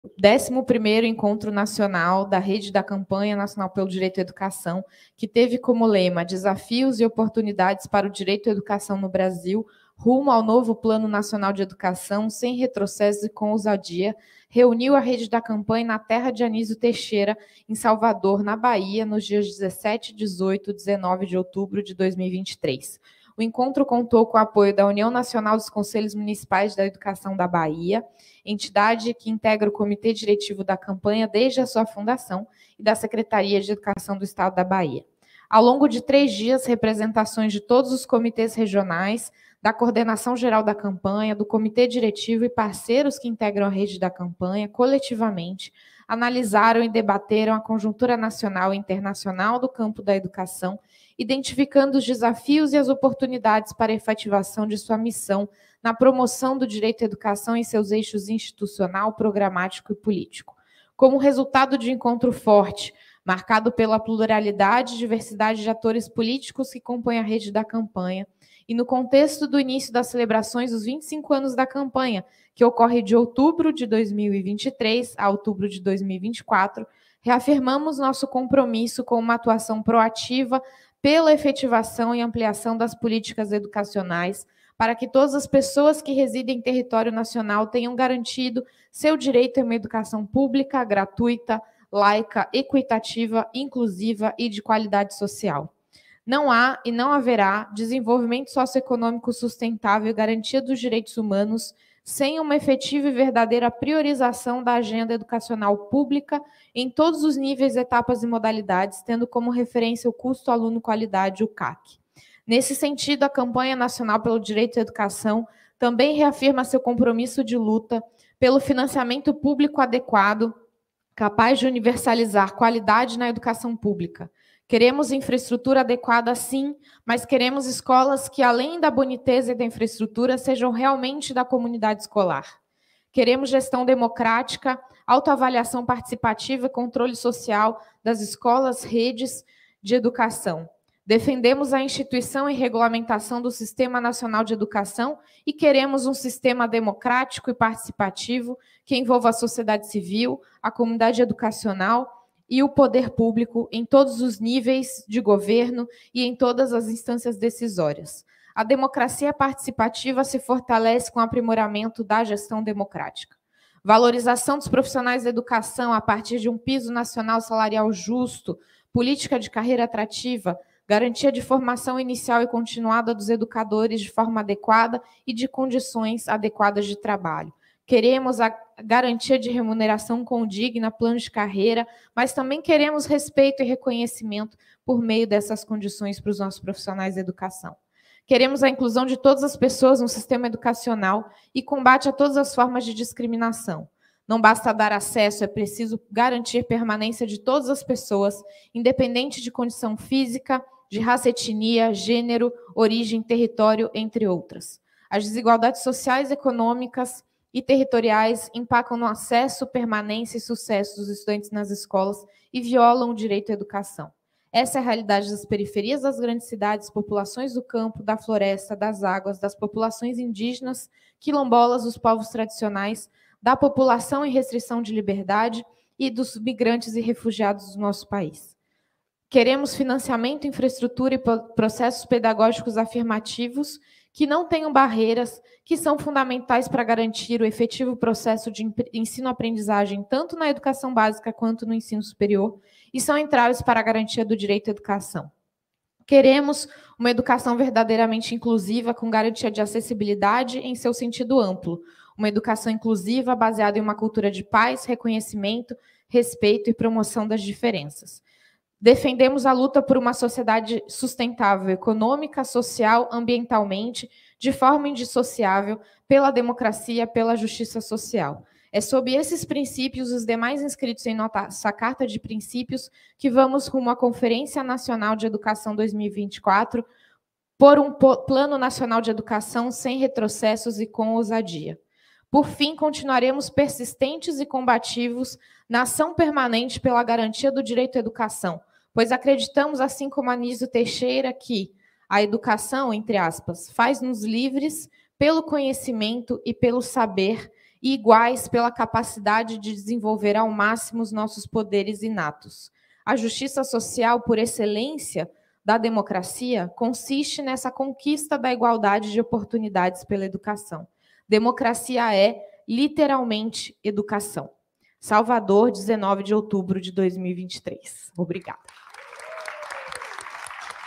11º ENCONTRO NACIONAL DA CAMPANHA PRODUZ A CARTA DE SALVADOR
O 11° Encontro Nacional da Campanha Nacional pelo Direito à Educação, que teve como lema “Desafios e oportunidades para o direito à educação no Brasil: Rumo ao novo PNE! Sem retrocessos e com ousadia!”, reuniu a Rede da Campanha na terra de Anísio Teixeira, Salvador (BA), nos dias 17, 18 e 19 de outubro de 2023.

O encontro contou com o apoio da União Nacional dos conselhos Municipais de Educação (Ucme-BA) -- entidade que integra o Comitê Diretivo da Campanha desde a sua fundação -- e da Secretaria da Educação do Estado da Bahia.

"Como resultado de um encontro forte, marcado pela pluralidade e diversidade de atores políticos que compõem a Rede da Campanha e, no contexto das celebrações dos 25 anos da Campanha que ocorre de outubro de 2023 a outubro de 2024, reafirmamos nosso compromisso com uma atuação proativa pela efetivação e ampliação das políticas educacionais para que todas as pessoas que residem em território nacional tenham garantido seu direito a uma educação pública, gratuita, laica, equitativa, inclusiva, e de qualidade social", diz a carta.